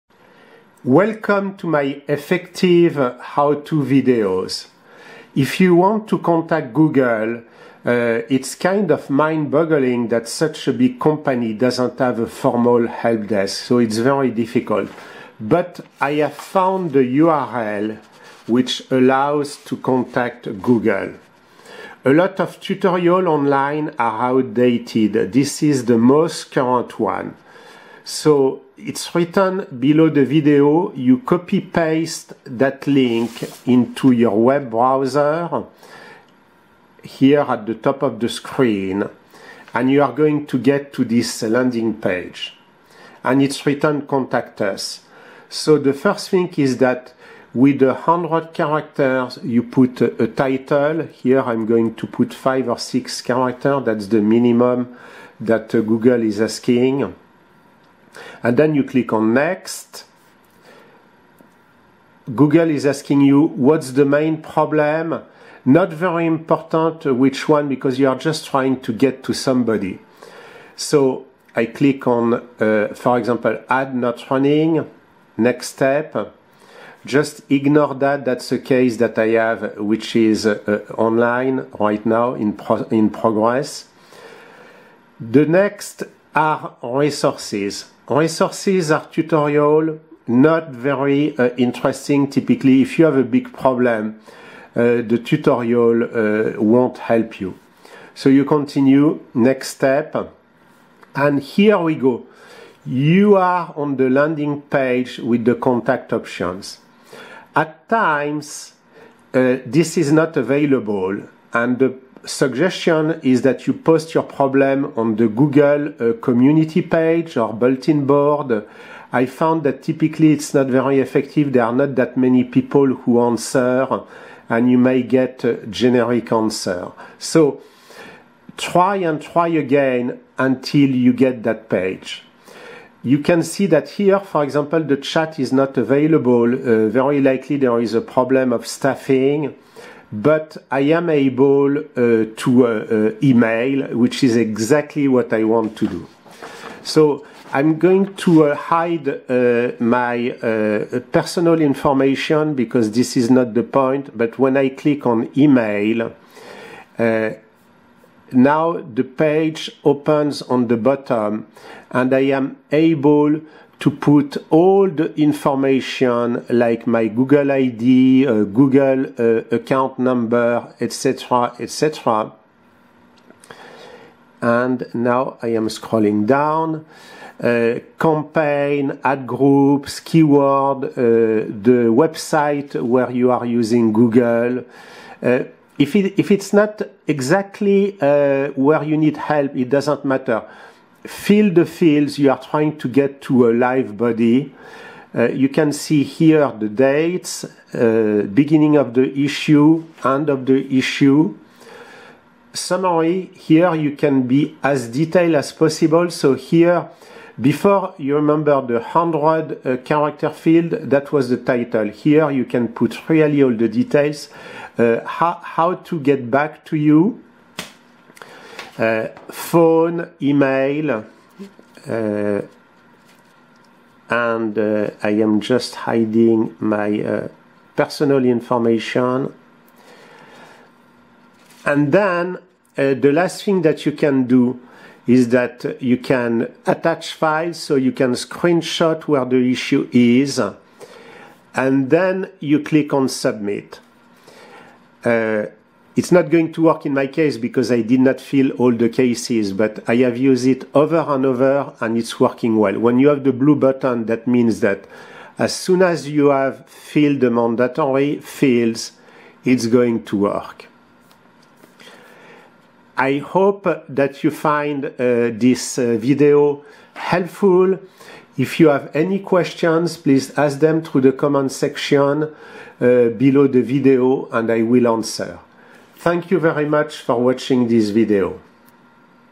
How to Contact Google Youtube Phone Chat Email Help Desk Works Successful
How to contact Google by phone / chat / email. It works. Be patients, Google opens and closes these functions many times a day.
I update regularly these links.
Reference: how to 049